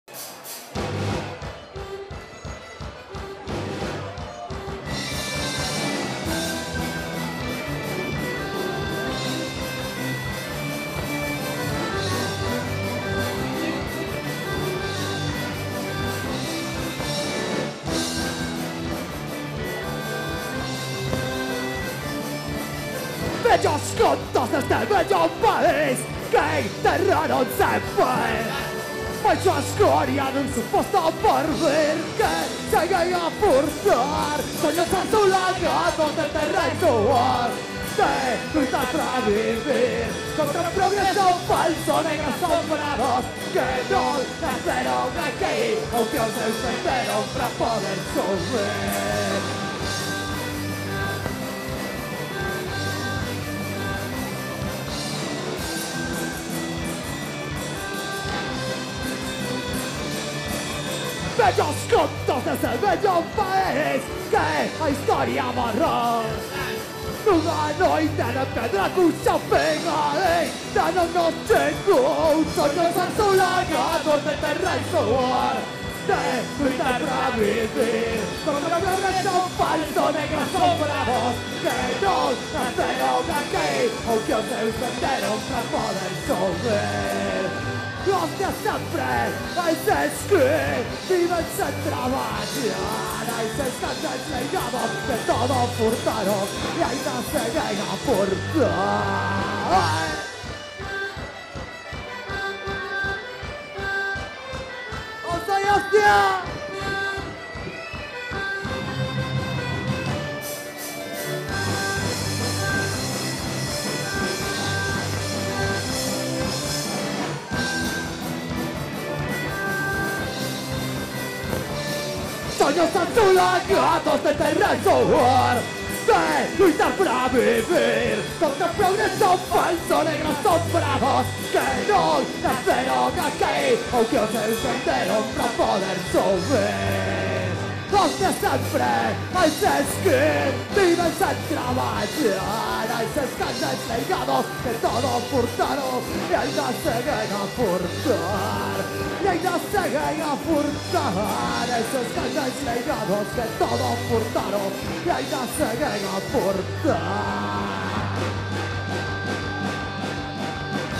TAÍÑA 2019 TECOR SOCIETARIO
Anaquiño do concerto de Tecor Societario, fechando a edición 2019 do Festival Taíña en Goián.